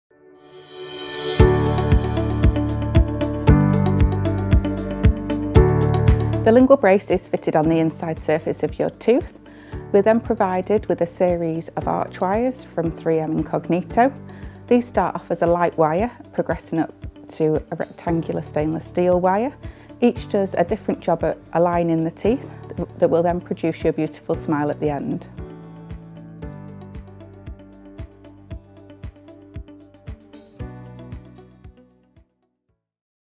How Do Lingual Braces Work To Align Teeth? | Northenden House Orthodontics
Lingual braces are a unique form of orthodontic treatment.
Watch the video to find out how they actually work.

Instagram - @manchesterorthodonticsCeramic Braces vs Metal Braces.